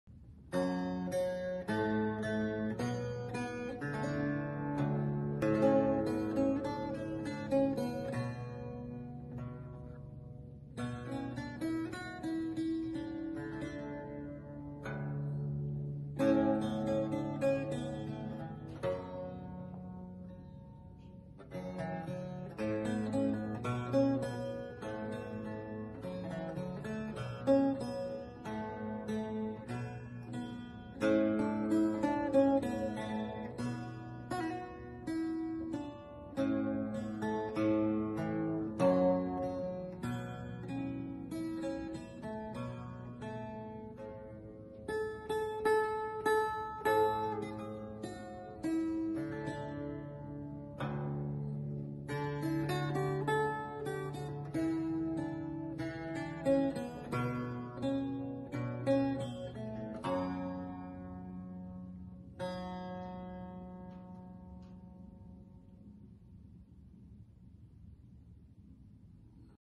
A port on wire-strung lute
A lute with wire strings
I have been playing the wire-strung instruments such as orpharion, cittern and English guittar, and I could not resist trying the wires on a standard lute.
We have very little historical information regarding the metal-strung lute: Praetorius talks about using wires on the lute in his Syntagma Musicum (1619/20) ending up with the re-entrant tuning like theorbo. There are several surviving liuto(s) converted for the wire strings.
In my experiment, I started with 0.13 Malcom Rose’s historical iron and “set up the Treble, so high as you dare venter for breaking”, followed by 0.19 iron for the second, Malcolm’s English brass strings for the middle and self-made twisted brass for the basses. I regulated the bridge and the nut to accommodate the wires. The tension and pitch are much lower than the normal gut-strung instrument anyway.
The result was quite interesting and I learned a lot. I will be writing a detailed report with more information for the Lute News or so.